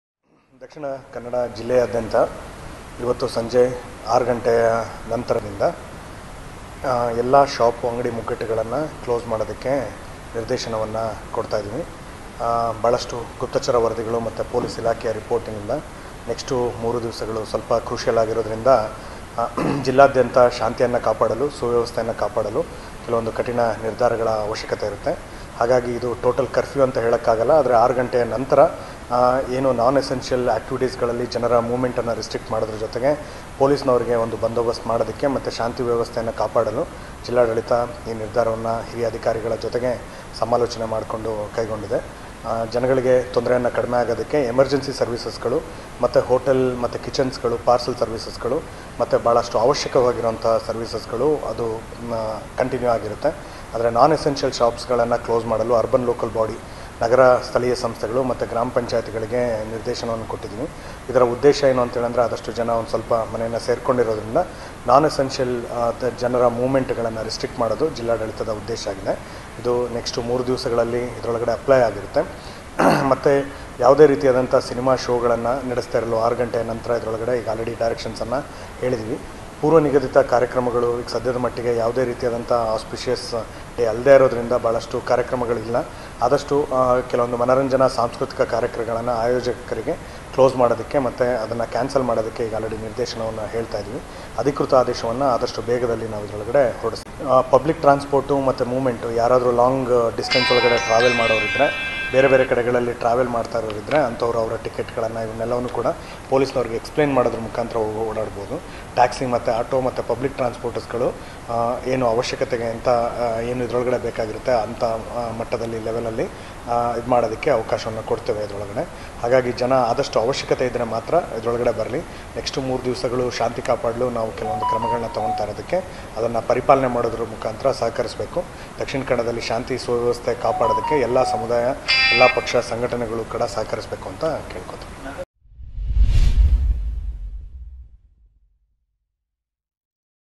ಸಂಜೆ 6ರ ಬಳಿಕ ದಕ್ಷಿಣಕನ್ನಡ ಜಿಲ್ಲೆಯ ಎಲ್ಲಾ ಅಂಗಡಿಗಳು ಬಂದ್ |Dakshinakannada DC orders closure of shops
ಮಂಗಳೂರು : ದಕ್ಷಿಣ ಕನ್ನಡ ಜಿಲ್ಲೆಯಲ್ಲಿ  ಸರಣಿ ಹತ್ಯೆಗಳ ಹಿನ್ನೆಲೆಯಲ್ಲಿ ಜಿಲ್ಲಾಡಳಿತವು ಆಗಸ್ಟ್ 1 ರವರೆಗೆ ತಕ್ಷಣದಿಂದ ಜಾರಿಗೆ ಬರುವಂತೆ ಸಂಜೆ 6 ರಿಂದ ಬೆಳಗ್ಗೆ 6 ರವರೆಗೆ ಎಲ್ಲಾ ಅಂಗಡಿ-ಮುಂಗಟ್ಟುಗಳನ್ನು ಮುಚ್ಚುವಂತೆ ಘೋಷಿಸಿದೆ.

For more..